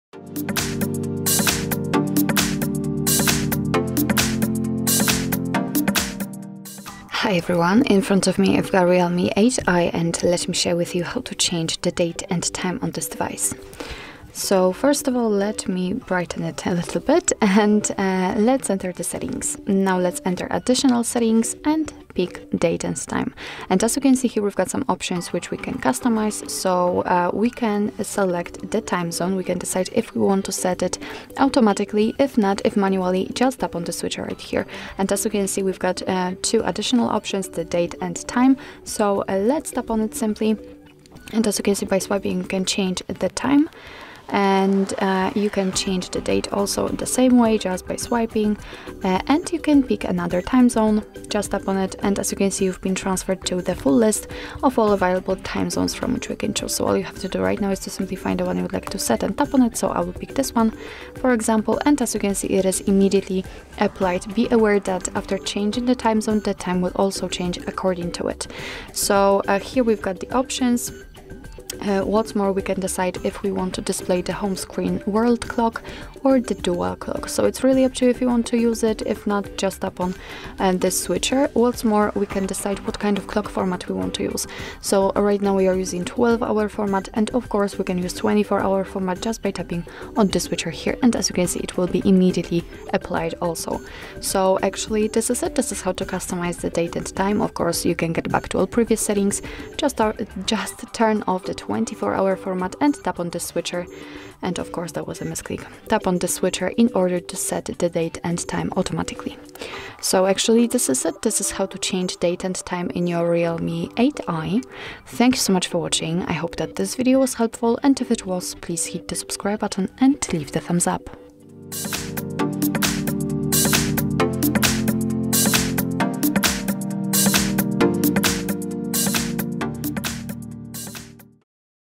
How to Change Date and Time on REALME 8i – Set Date & Time
REALME 8i Tutorials:
If your REALME 8i displays the wrong date and/or time, you can easily change that and set the right one. Watch this video tutorial to find how where you can find the date & time settings where you will be able to set the correct time. Follow the instructions mentioned by our expert to easily change the date and time on your REALME 8i. Check out HardReset.info YouTube channel if you wish to make more changes or adjustments to your REALME 8i.